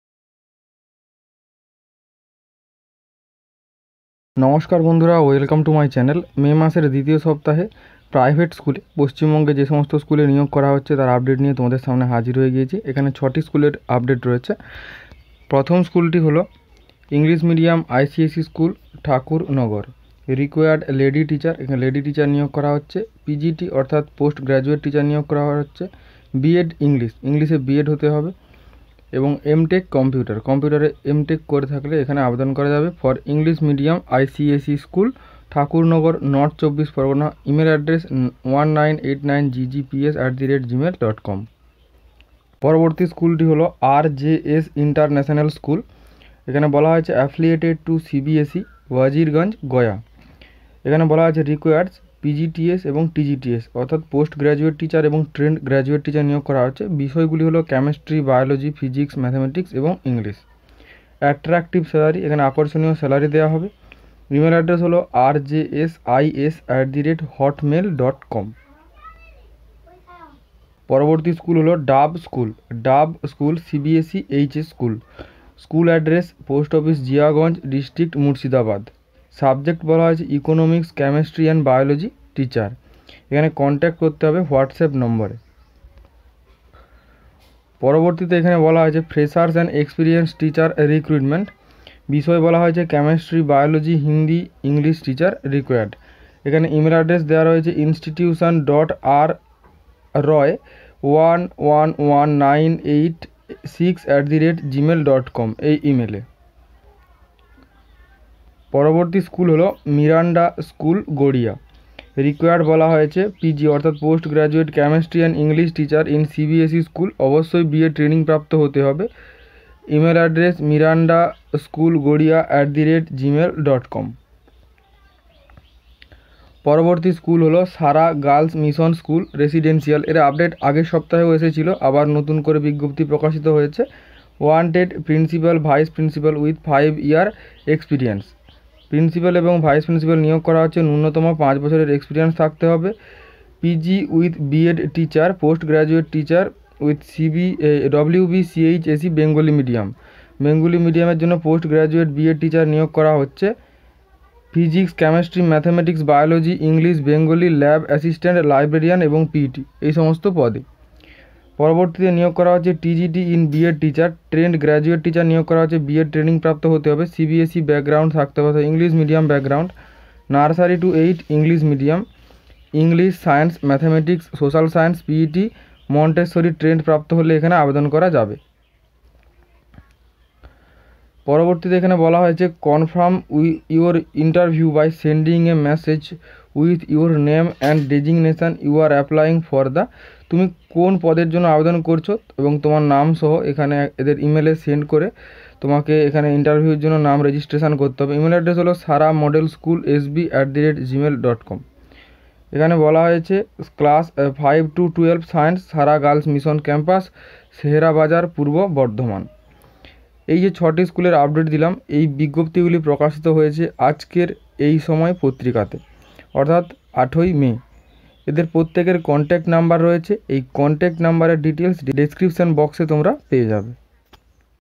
পশ্চিমবঙ্গে শিক্ষক নিয়োগ 2022 | West Bengal Teacher Recruitment 2022 | Private School Job
In this channel I upload govt jobs and private Jobs scheme scholarship related videos.

Today I Discuss about Miranda School, dav school, sara girls school, rjs international school teacher recruitment notification 2022

• English Medium ICSE School, Thakurnagar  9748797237
• RJS International School Cont. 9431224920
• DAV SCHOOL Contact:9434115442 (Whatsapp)
• miranda school What's App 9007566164
• Sara Girls Mission,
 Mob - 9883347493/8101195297
• all job notification Published On Ei Samay Paper 08/05/2022